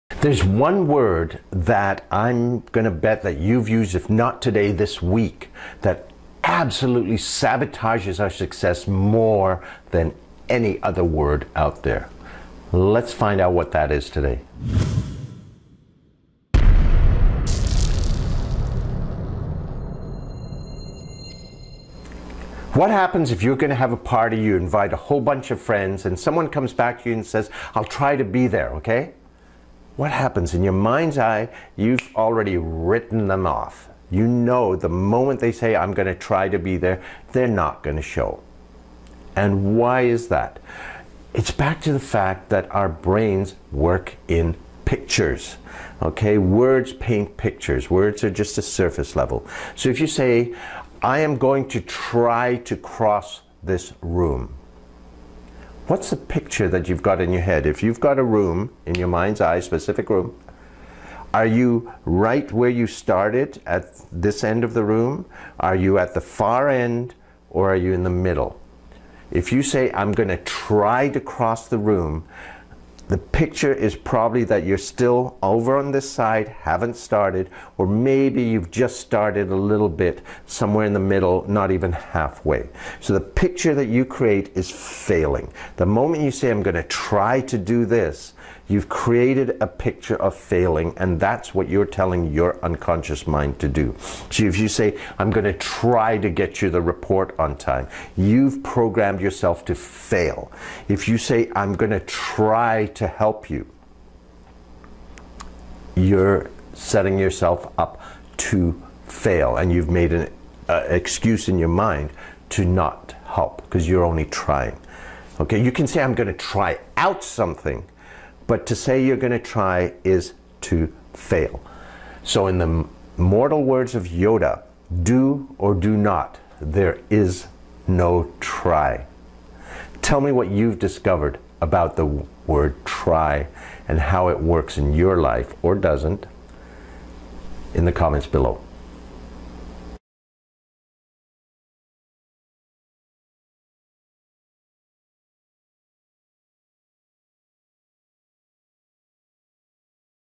One word that sabotages you more than any other
There's one word that I'm going to bet that you've used, if not today, this week, that absolutely sabotages your success more than any other word you use.